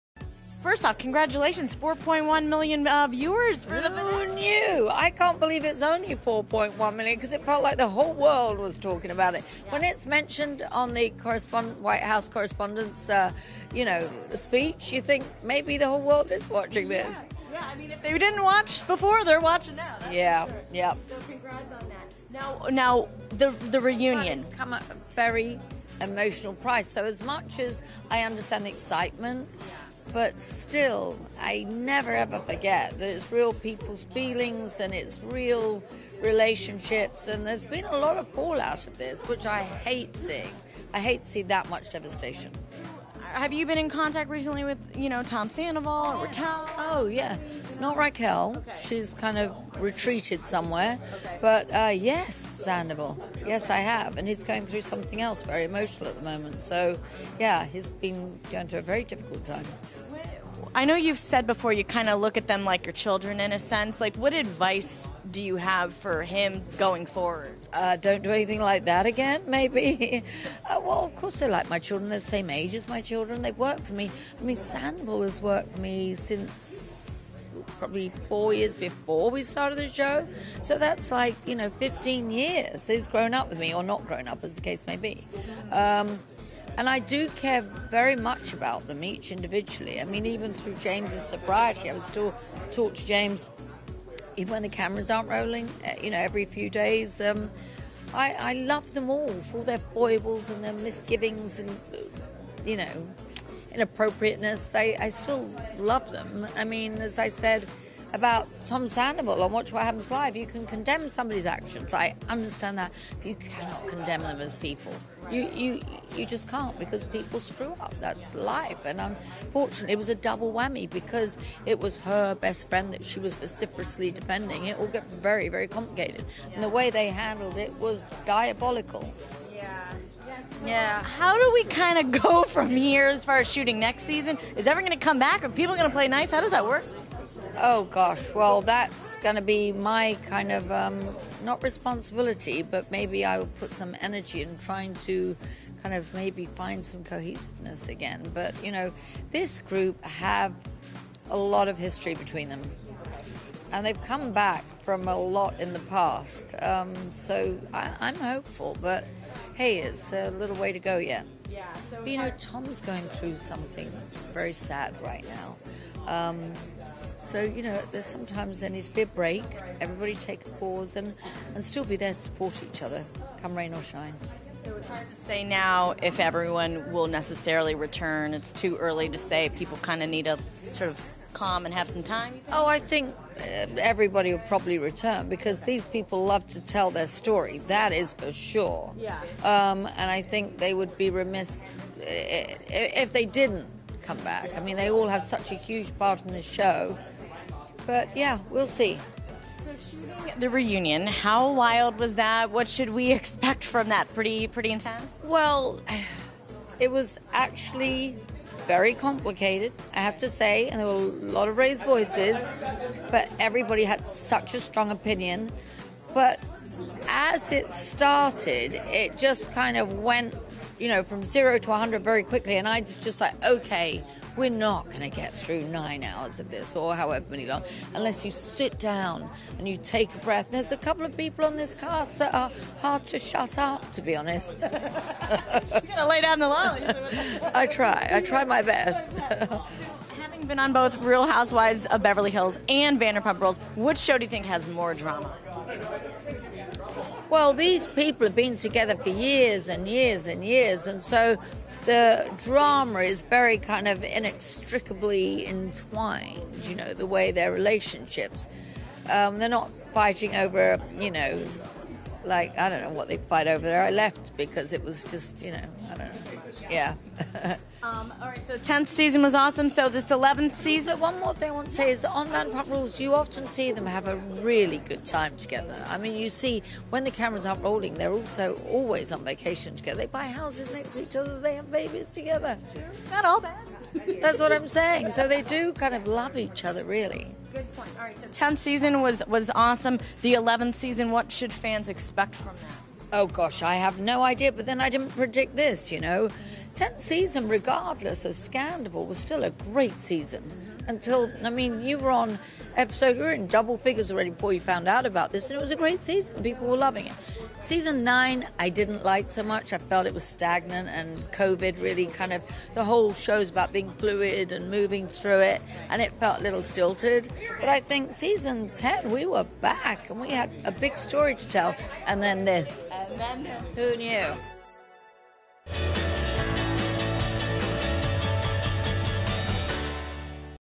Lisa Vanderpump Reveals ADVICE She Gave Tom Sandoval Amid Scandal | E! News
Lisa Vanderpump weighs in on the Tom Sandoval and Raquel Leviss cheating scandal and shares the advice she gave Tom. Plus, she talks "Vanderpump Rules" reunion and more!